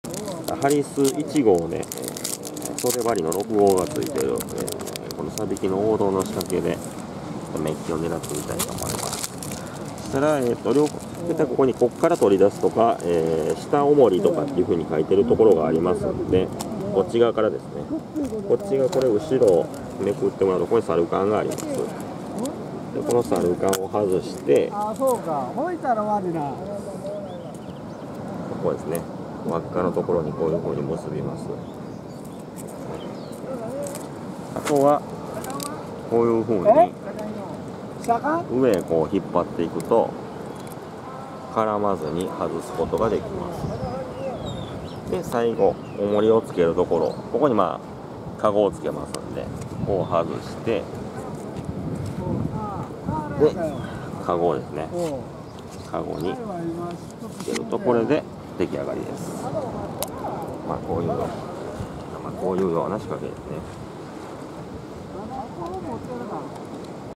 サビキ仕掛けのセットの仕方 【レッツ・ゴー・フィッシング】
サビキ仕掛には取り出し口（ここから取り出す、下オモリ等）の表示があることが多いので、お間違いの無き用に。
反対に取り付けると針の向きが下を向き、魚がヒットし難い状態になります。
セットしたら仕掛のクセが付いているので、引っ張って糸ヨレを取り除きましょう。